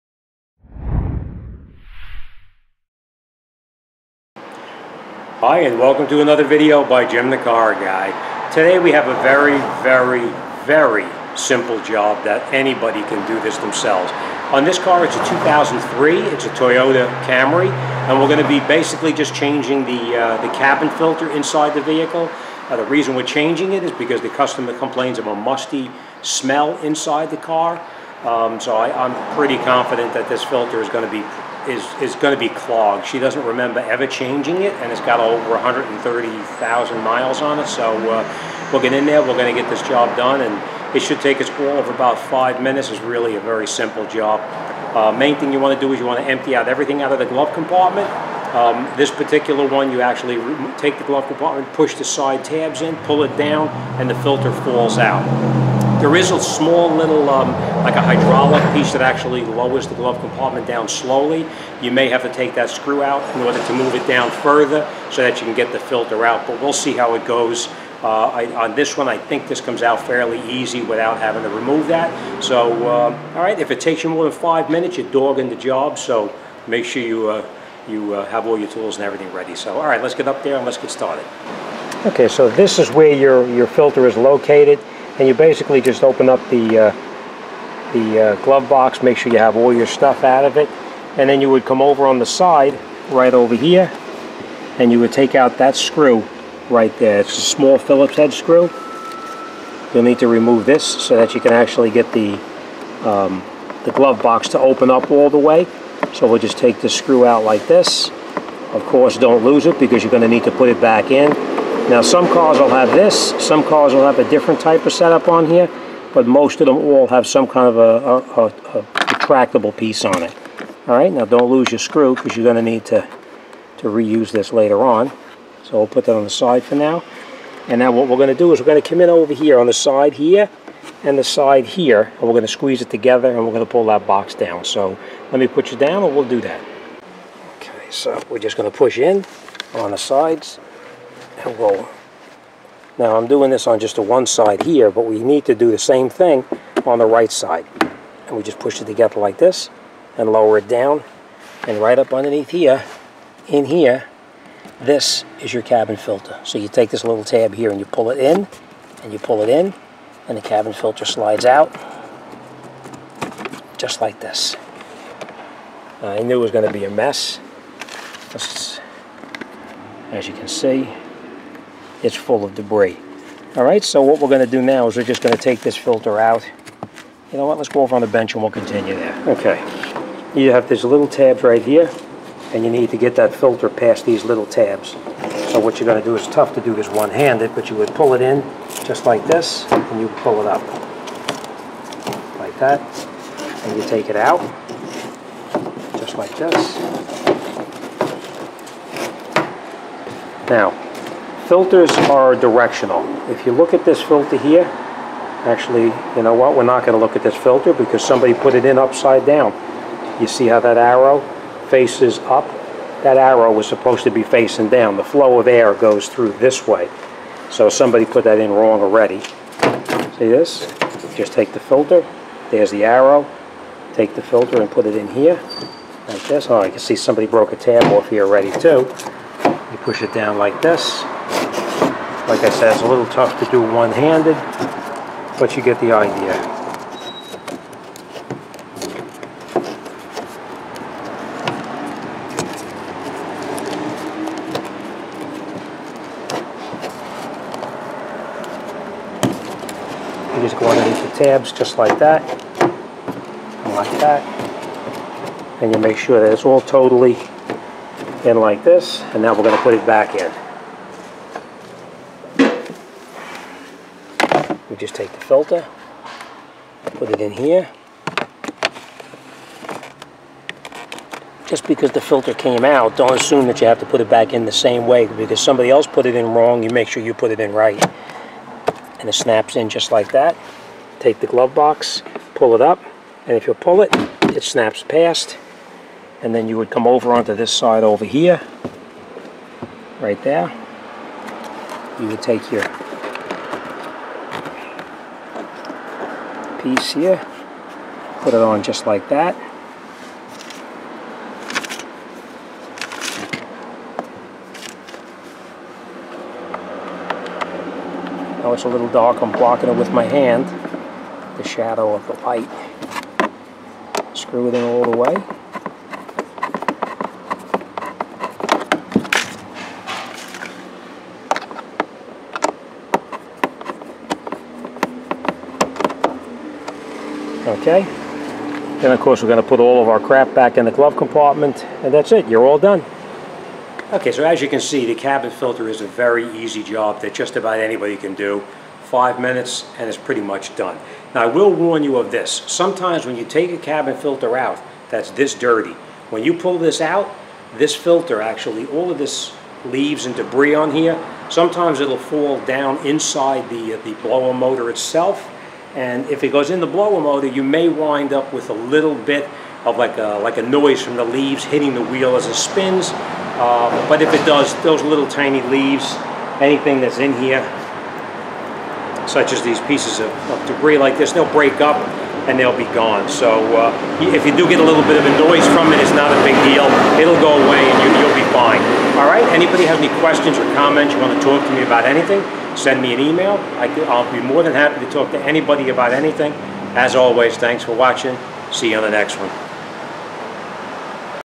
How to replace the cabin filter 2002-2009 Toyota Camry
I will show you how to replace the cabin filter on this 2003 Toyota Camry.This job is very easy.it should be finished in less than 5 minutes